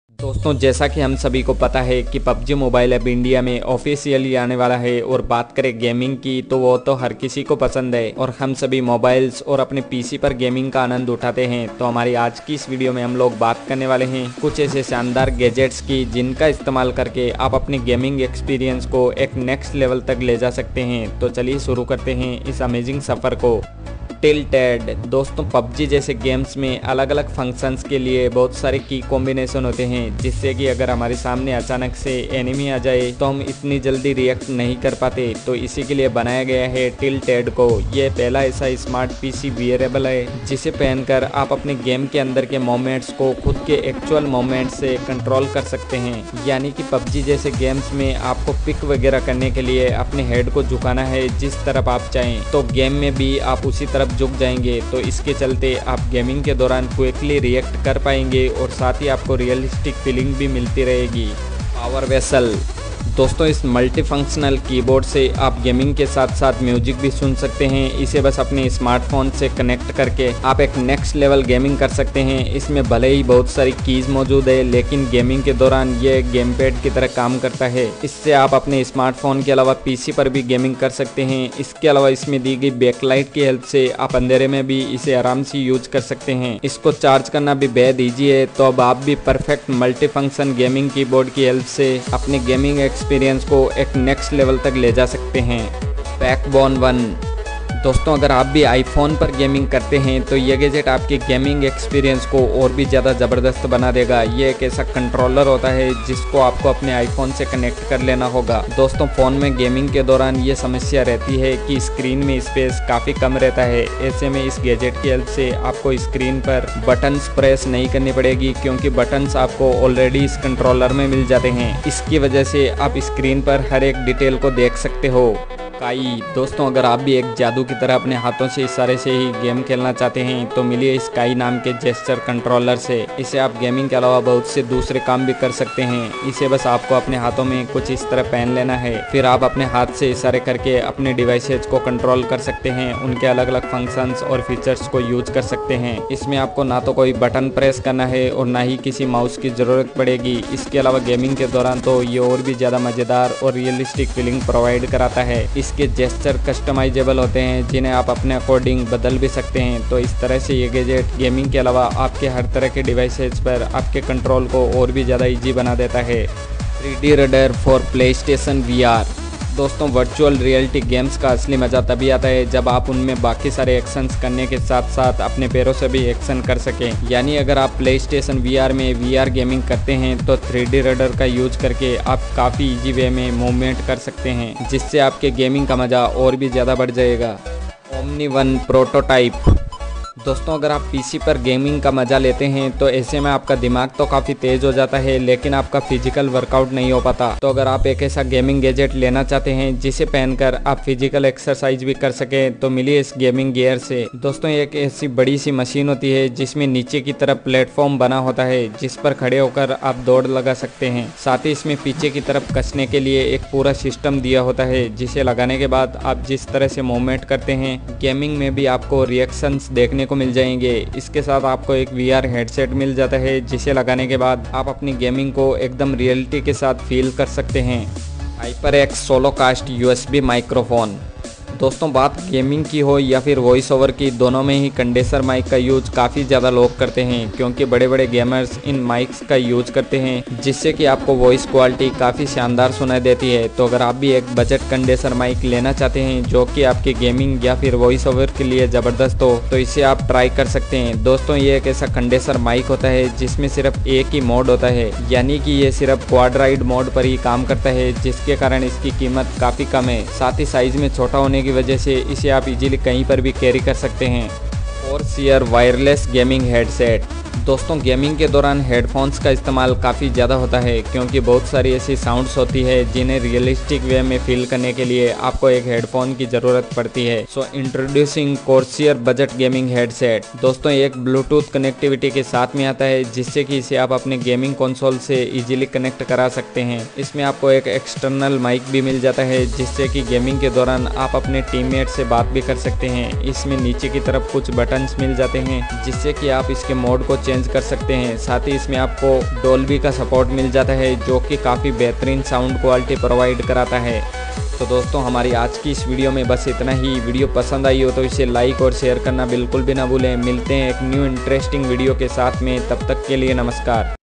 NOW PLAY LIKE A PRO | 8 GAMING GADGETS FREEFIRE & PUBG | GADJETS UNDER RS.99 , RS.199, RS.499
today, we're rounding up our best gaming gadgets of 2021. Because if you're at home instead of the beach, you might as well treat yourself to some new gaming gear.
Like every gamer, you want to improve your gaming skills, but don't know how right? If you use an old setup, you need to upgrade it first. Finding the best gears makes your gaming experience better.